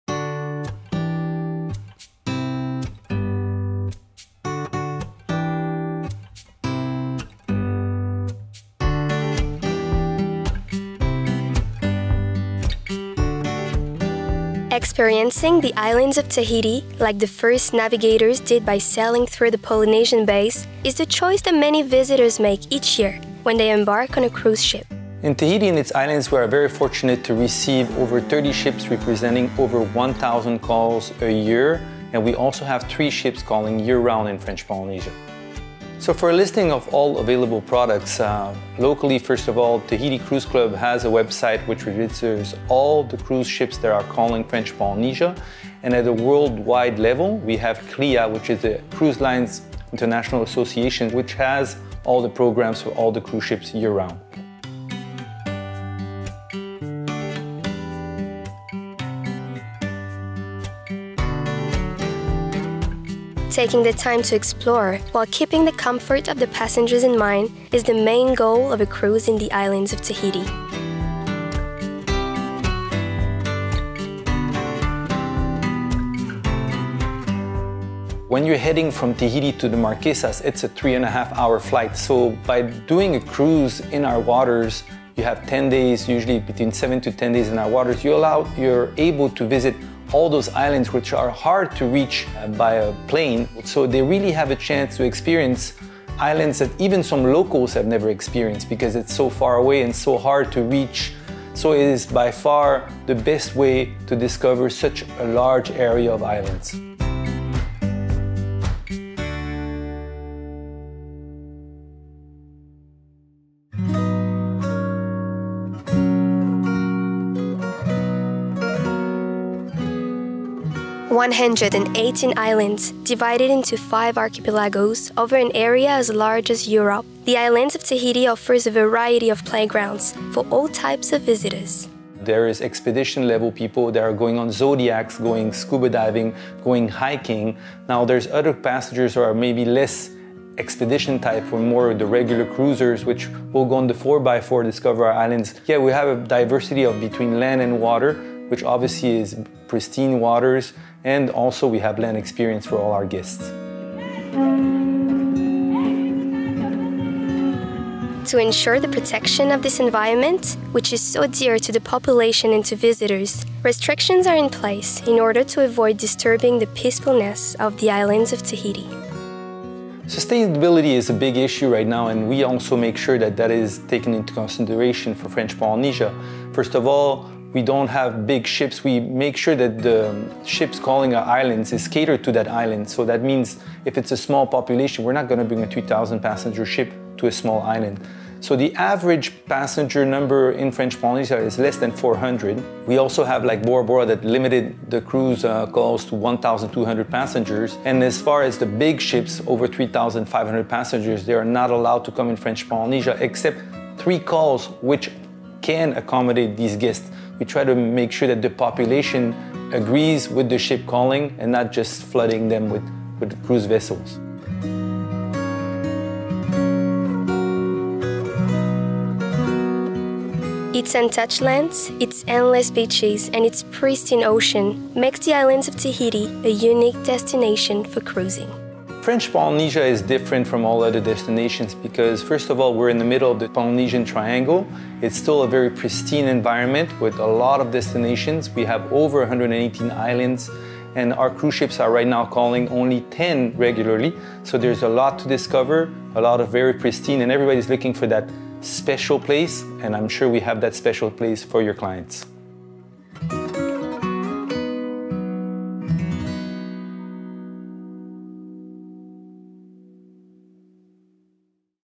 Cruising In The Islands of Tahiti 2023
Several prestigious cruise companies visit The Islands of Tahiti with some of jewels of their fleets. With numerous ports of call in the islands, it is the perfect way to discover French Polynesia at a pleasant pace and in total luxury.